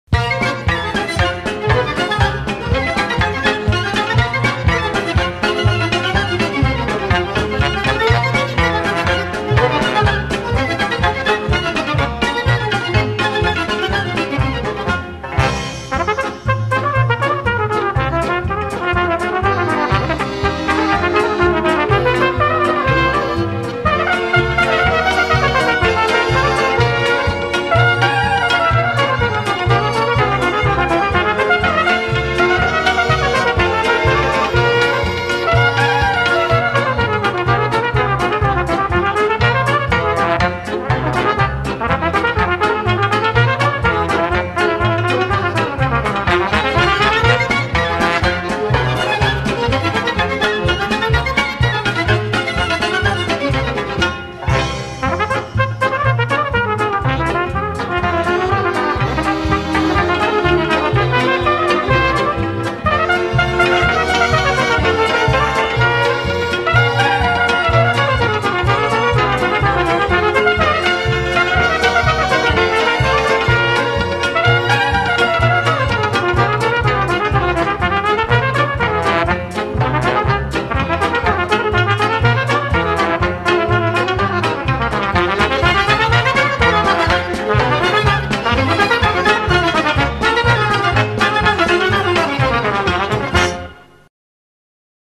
Music from Bucovina Region
Most Pictures from Bucovina (ukrainian part),
played by ukrainian Musicians.